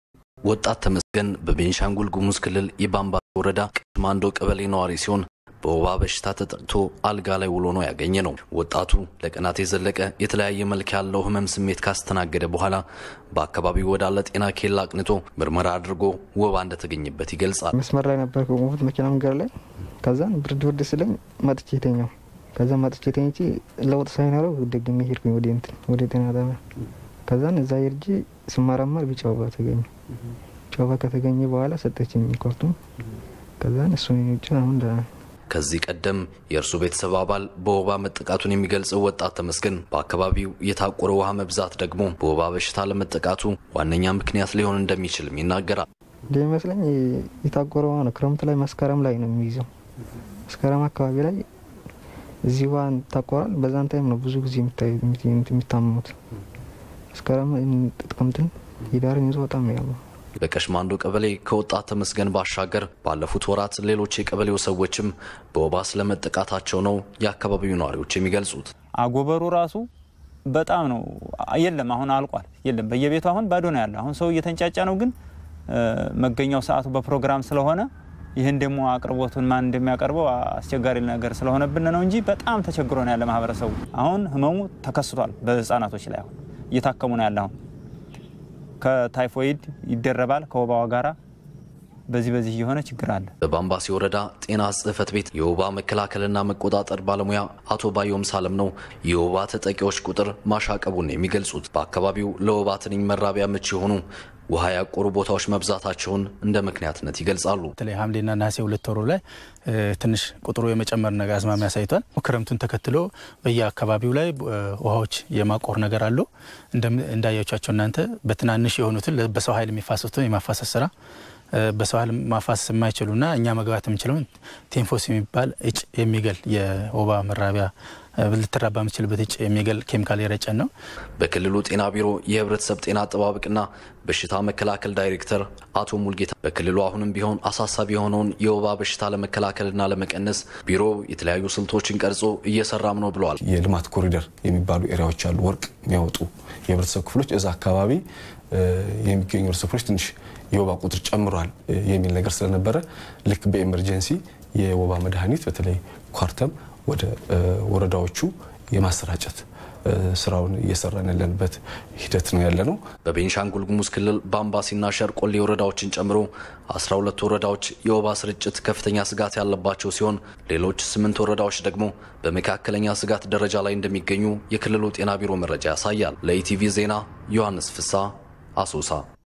በቤኒሻንጉል ጉሙዝ ክልል የወባ በሽታ ስርጭት መጠኑ ክፍተኛ ነው ተባለ፡፡
etv በቤኒሻንጉል ጉሙዝ ክልል የወባ በሽታ ስርጭት መጠኑ ክፍተኛ ነው ተባለ፡፡